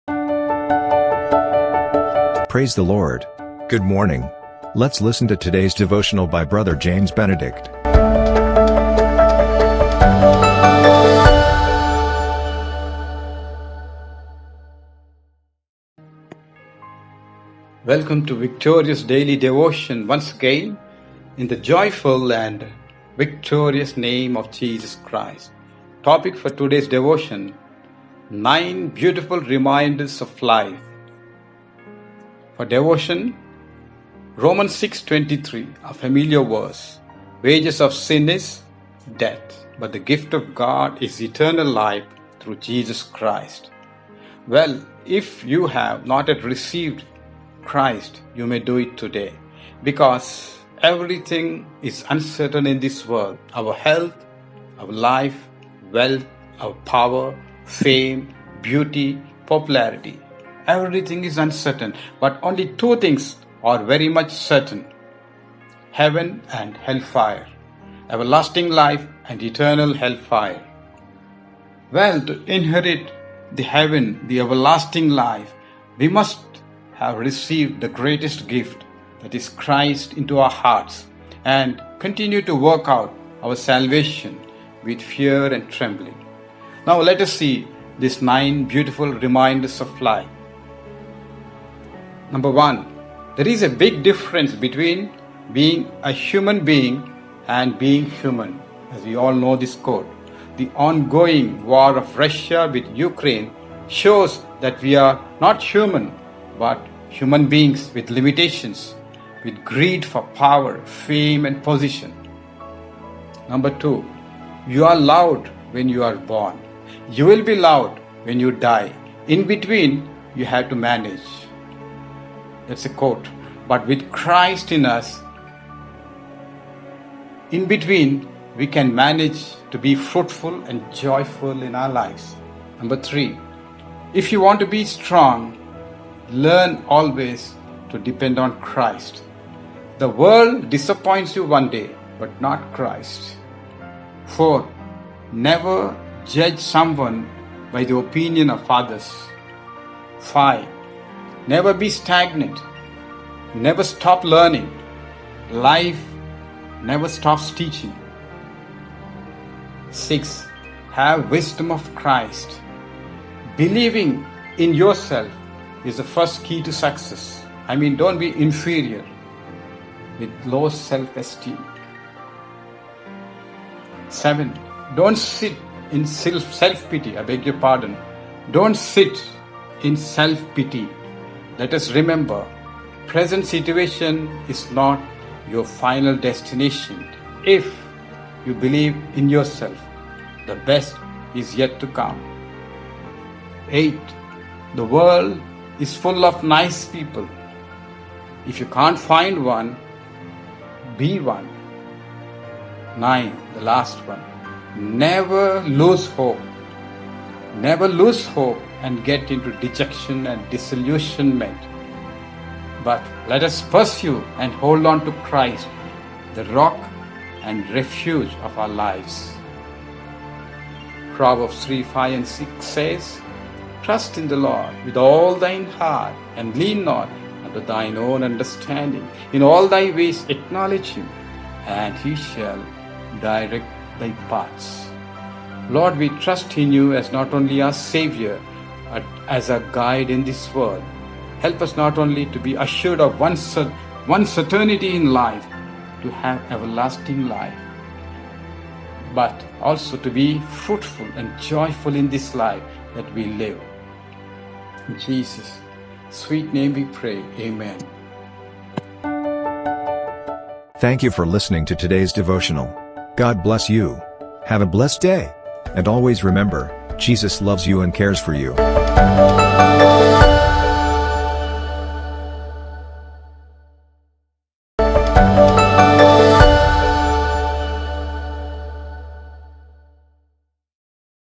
Victorious Daily Devotional | March 1, 2022 | Ps. Benedict James | 9 Beautiful Reminders Of Life
Welcome to "Victorious Daily Devotion" in the Victorious and Joyful name of Jesus Christ! 💐🙂🌹

Topic : Nine Beautiful  Reminders Of Life:

Rom. 6:23. Wages of sin is death; but the gift of God is eternal life through Jesus Christ.

If you have not yet received Christ, you may do it today. Because everything is uncertain in this world - our health, our life, our wealth, our power, our fame, our beauty, our pupularity...  But two things are very much certain, Heaven and hell fire.  Everlasting Life and Eternal Hell fire.

To inherit the everlasting life, we must hace received the gift, Christ into our hearts, and continue to work out our salvation with fear and trembling.

Nine Beautiful Reminders Of Life:

1. There is a big difference between being a "human being"  and being "human"

The ongoing war of Russia with Ukraine shows we are not human but human beings with  greed for  power, fame and  position.

2. "You are loved when you are born. You will be loved when you die. In between you have to manage."
But with Christ in us it can be fruitful and joyful.

3. If you want to be strong, learn always to depend on Christ. The world disappoints you one day, but not Christ.

4. Never judge someone by the opinion of others.

5. Never be stagnant. Never stop learning. Life never stops teaching.

6. Having wisdom of Christ. Believing  in yourself is the first key to success. I mean, don't be inferior.

7. Don't sit in self pity. Let us remember, "Present situation is not your final destination, if you believe in yourself." The best is yet to come."

8. The world of full of nice people. If you can't find one, BE one. 🙂.

For complete devotion, let us watch this short video.

Have a blessed and joyful day!! 🙋💐🙂

Benedict James.